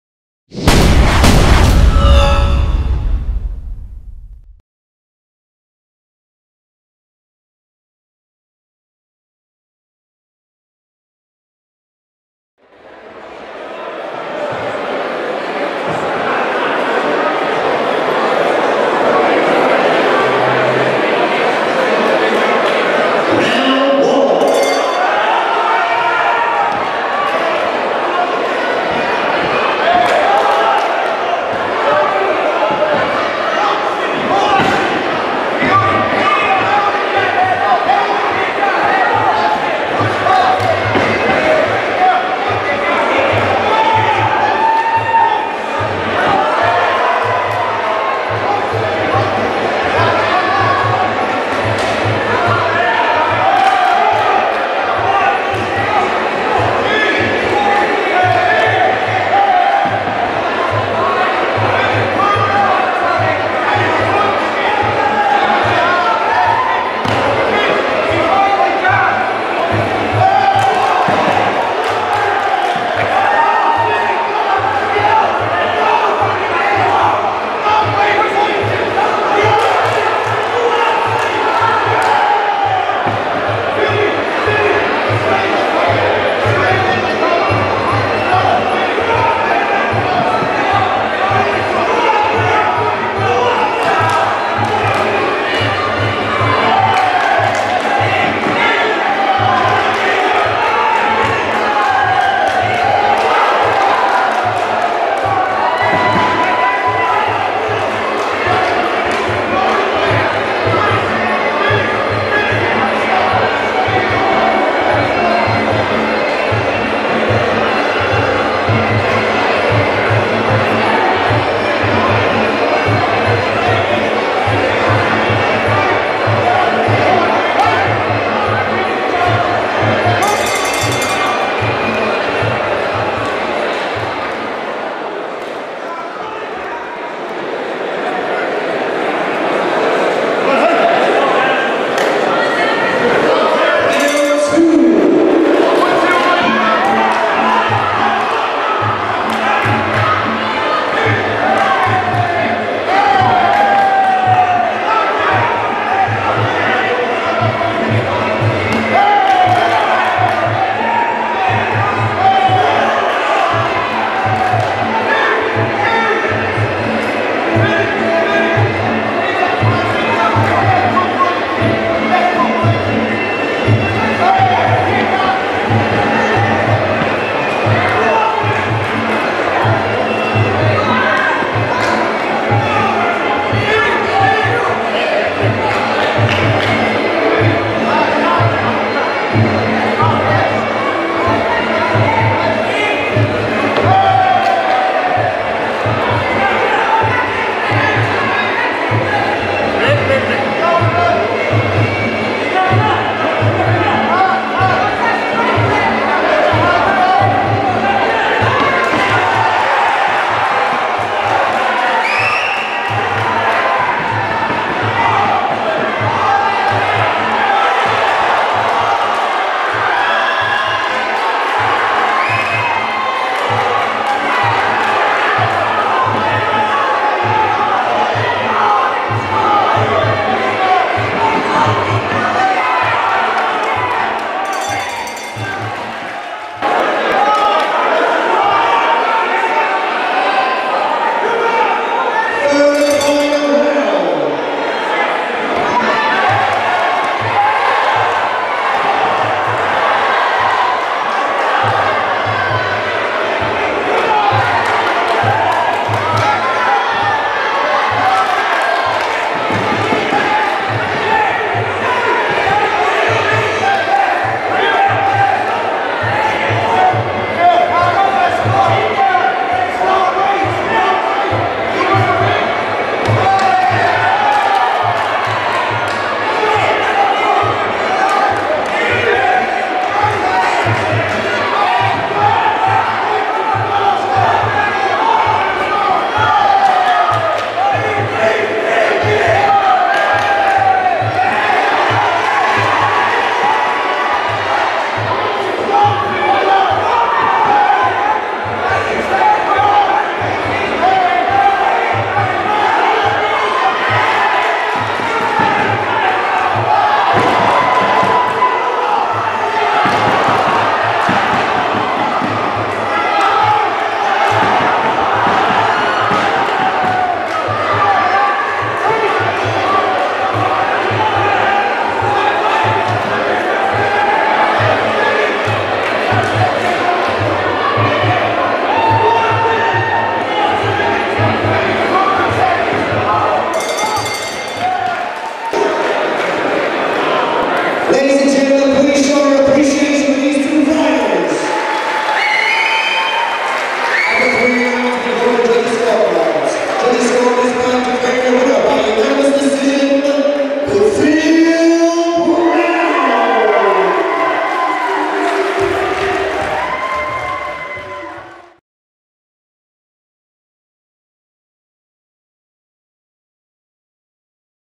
Darren Brennan v Phil Brown - The Takeover 9
Darren Brennan (Warriors) v Phil Brown (Jai Sua)
AM Thai - 88kg - 3x1.5min
The Takeover 9
Naomh Ólaf CLG, Dublin
Sat 10th June